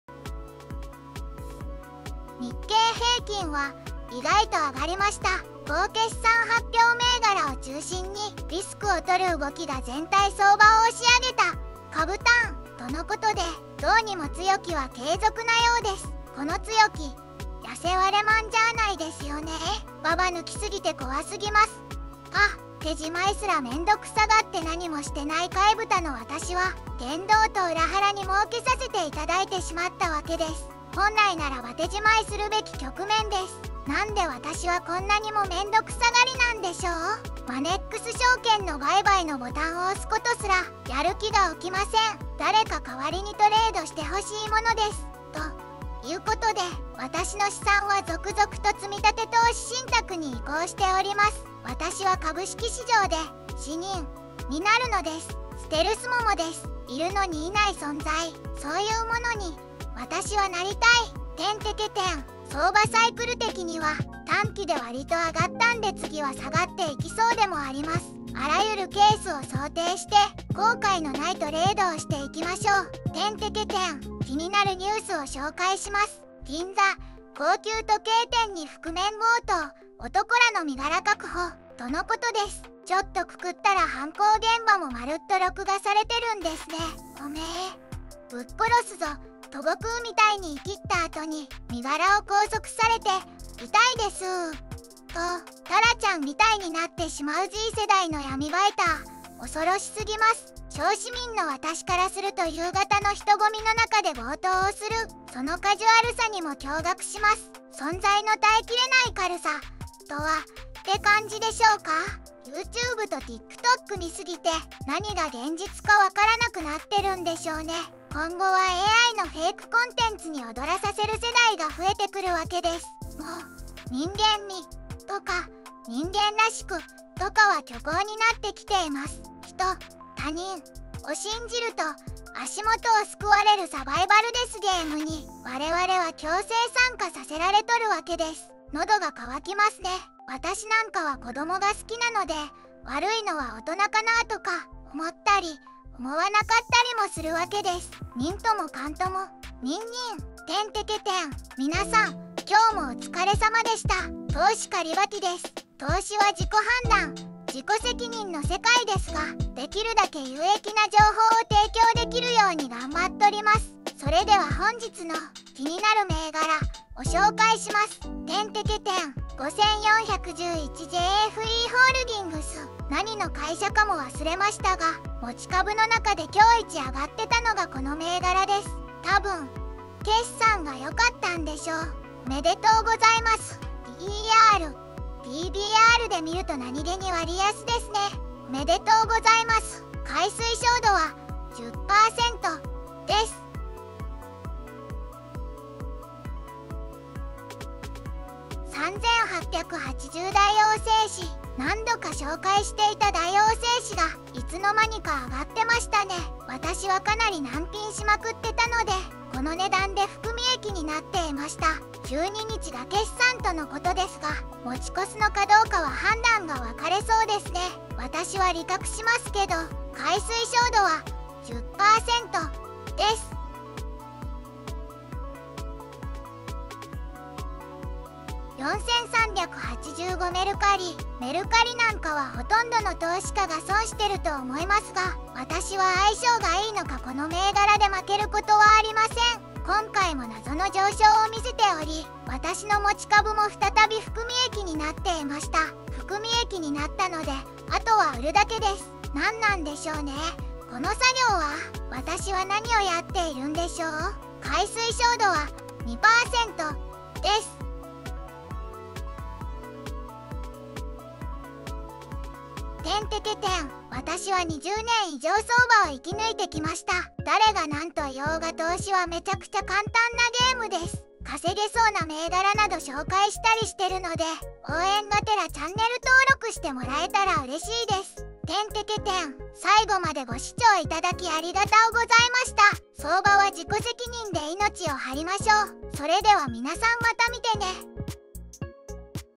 【23.5.9】本気でみんなを儲けさせようとする動画
日経平均は「意外と上がりました」
”好決算発表銘柄を中心に
リスクを取る動きが全体相場を押し上げた（株探）”
とのことで、
どうにも強気は継続なようです。。
この強気、、やせ我慢じゃあないですよね？？
ババ抜き過ぎて怖すぎます。が、
手仕舞いすらめんどくさがって何もしてないカイ豚の私は
言動と裏腹に儲けさせていただいてしまったわけです。
本来ならば手仕舞いするべき局面です。。
なんで私はこんなにもめんどくさがりなんでしょう？？
マネックス証券の売買のボタンを押すことすら
ヤル気が起きません。
誰か代わりにトレードしてほしいものです。
と、いうことで
私の資産は続々と積立投資信託に移行しております。
私は株式市場で「死人」になるのです。
ステルスモモです。
居るのに居ない存在。
そういうものに、私はなりたい。
相場サイクル的には、
短期でわりと上がったんで次は下がっていきそうでもあります。
あらゆるケースを想定して、
後悔の無いトレードをしていきましょう！
気になるニュースを紹介します
”銀座　高級時計店に覆面強盗　男らの身柄確保”
とのことです。
ちょっとググったら犯行現場もまるっと録画されてるんですね＾＾；
「オメェ、ぶっ殺すぞ」と悟空みたいにイキったあとに、
身柄を拘束されて「痛いですぅー」と
タラちゃんみたいになってしまうZ世代の闇バイター。。恐ろしすぎます。
小市民の私からすると夕方の人混みの中で強盗をする
そのカジュアルさにも驚愕します。。
「存在の耐えきれない軽さ」とは、って感じでしょうか？？
YOUTUBEとティックトック見過ぎて
何が現実か判らなくなってるんでしょうね。
今後はAIのフェイクコンテンツに踊らさせる世代が増えてくるわけです。
もう、「人間味」とか「人間らしく」とかは虚構になってきています。
ヒト（他人）を信じると、足元を掬われるサバイバルデスゲームに
我々は強制参加させられとるわけです。。
のどが渇きますね。
私なんかは子供が好きなので、悪いのは大人かなぁとか
思ったり、思わなかったりもするわけです。。
にんともかんとも、にんにん！
みなさん、今日もお疲れ様でした＾＾
投資家リバティです！
投資は自己判断・自己責任の世界ですが、
出来るだけ有益な情報を提供できるように頑張っとります
それでは本日の、
「気になる銘柄」を紹介します！
5411ＪＦＥホールディングス
何の会社かも忘れましたが、
持ち株の中で今日イチ上がってたのがこの銘柄です。
多分、決算が良かったんでしょう。
「オメデトウゴザイマス！」
PER・PBRで見ると何気に割安ですね。
カイ推奨度は「10％」です
3880大王製紙
何度か紹介していた大王製紙が
いつの間にか上がってましたね。
私はかなりナンピンしまくってたので、
この値段で含み益になっていました。
12日が決算とのことですが
持ち越すのかどうかは判断が分かれそうですね。
私は利確しますけど。
4385メルカリ
メルカリなんかは殆どの投資家が損してると思いますが、
私は相性がいいのかこの銘柄で負けることはありません。
今回も謎の上昇を見せており、
私の持ち株も再び含み益になっていました。
含み益になったので、あとは売るだけです。
なんなんでしょうね？この作業は？？
私は何をやっているんでしょう？？
カイ推奨度は「2％」です
私は20年以上相場を生き抜いてきました。
誰が何と言おうが投資は滅茶苦茶簡単なゲームです。
稼げそうな銘柄など紹介したりしてるので、
最後までご視聴いただき有難うございました
相場は自己責任で命を張りましょう。
それではみなさんまた見てね！